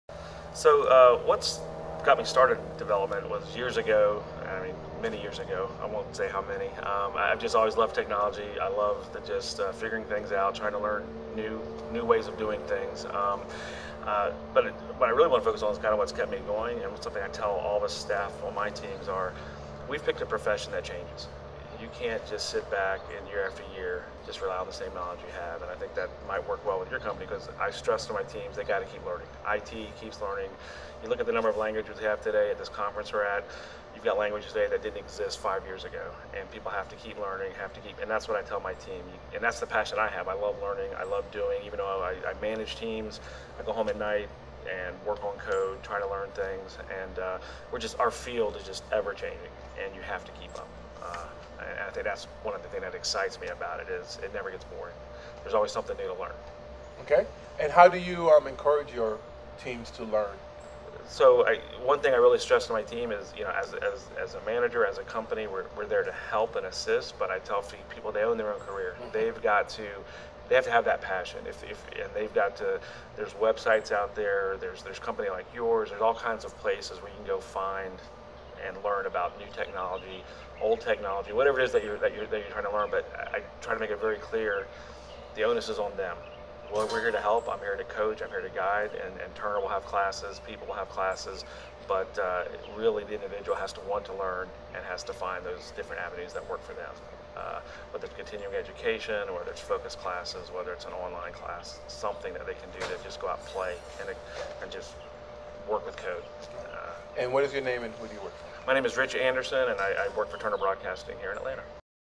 Atlanta Code Camp - Rich Anderson - Own Your Own Career
Rich Anderson shares how he got started as a software programmer, what keeps him going through his career and offers some advice to those that are in software development.